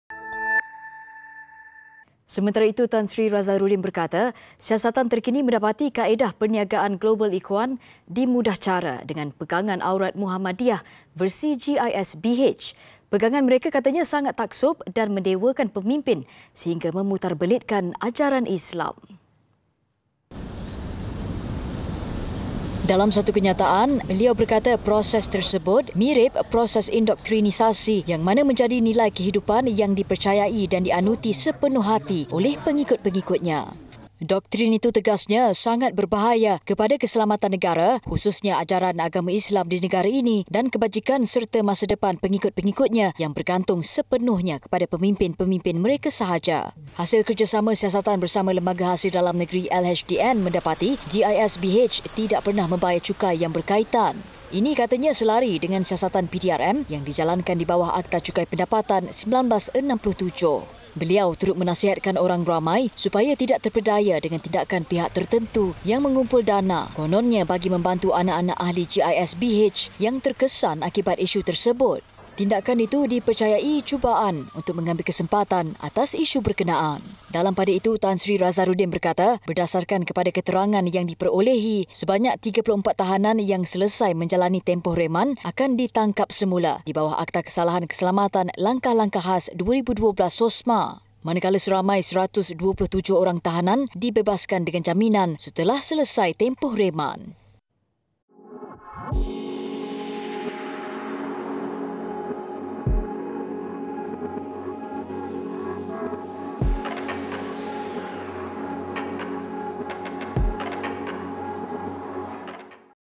KES GISBH | Pegangan Pengikut Sangat Taksub Dan Mendewakan Pemimpin
Tan Sri Razarudin berkata siasatan terkini mendapati kaedah perniagaan Global Ikhwan dimudahcara dengan pegangan Aurad Muhammadiah versi GISBH.